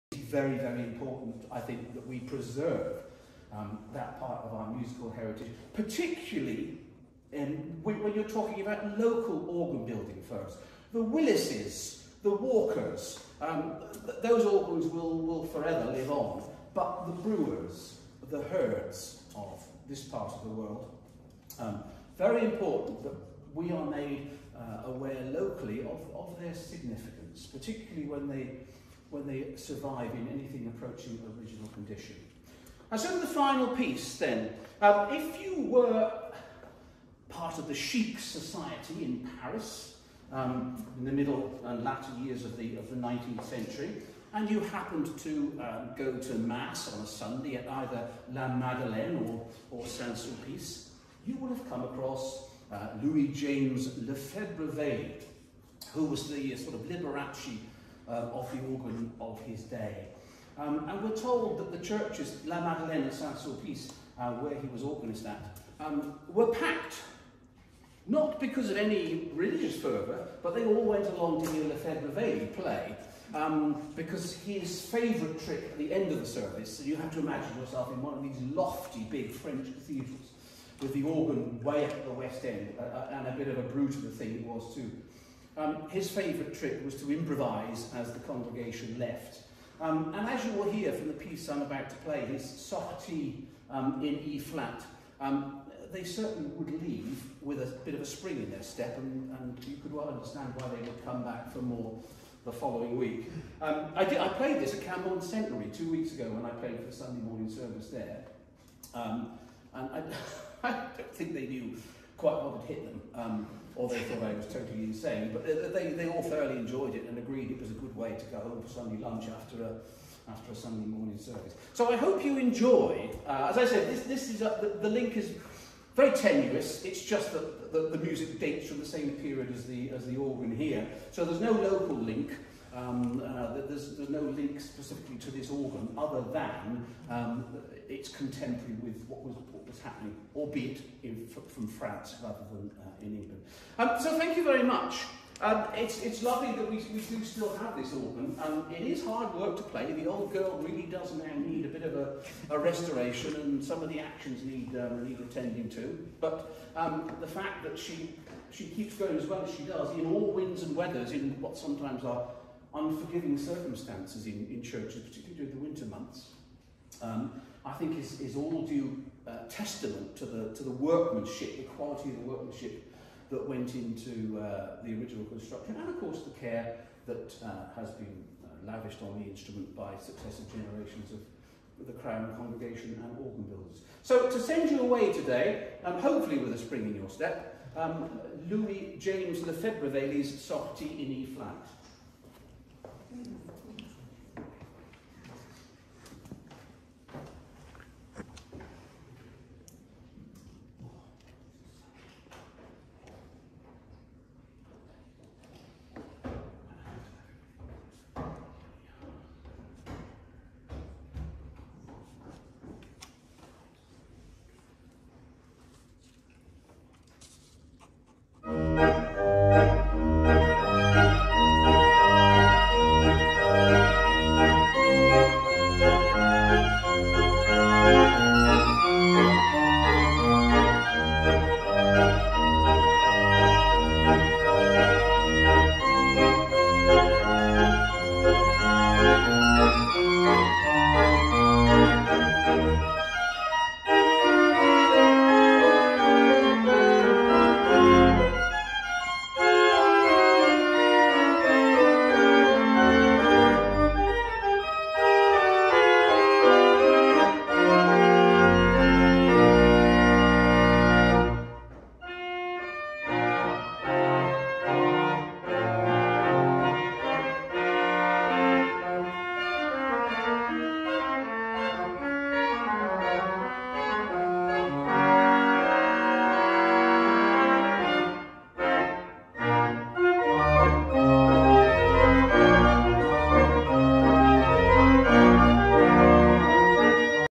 Organist Jeffrey Williams at Crowan Church June 2021
Organist Jeffrey Williams speaking on 20 June 2021 at the first of three summer concerts at Crowan Church, Cornwall. The concert was to celebrate the organ, given by Caroline Molesworth St Aubyn (nee Wheler) in memory of her parents Rev. Charles and Anne Wheler of Ledston Hall, Yorkshire, and Otterden Place, Kent.  The organ was inaugurated in May 1891 and Jeffrey played some of the pieces performed that day by the organist of Truro Cathedral, Dr. Mark James Monk, who travelled down to Crowan to play.